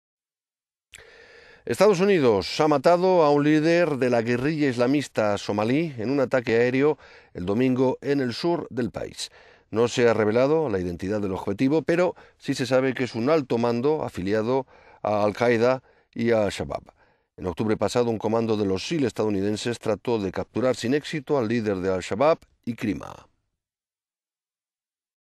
EEUU mata a un líder guerrillero somalí en un ataque aéreo
Estados Unidos ha matado a un líder de la guerrilla islamista somalí en un ataque áereo el...

Estados Unidos ha matado a un líder de la guerrilla islamista somalí en un ataque áereo el domingo en el sur del país. En octubre pasado, un comando de los SEAL estadounidenses trató de capturar sin éxito al líder de Al-Shabaab,  Ikrima.